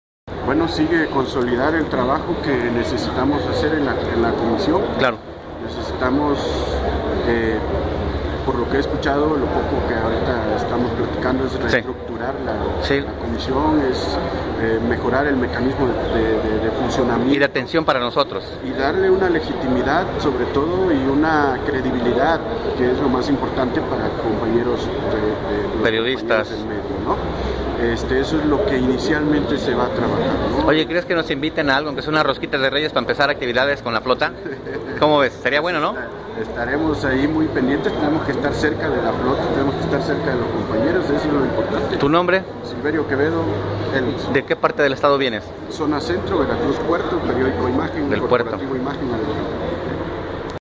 Silverio Quevedo
Al termino de la toma de protesta en el Congreso de Veracruz a Comisionados en la atención y protección a periodistas CEAPP.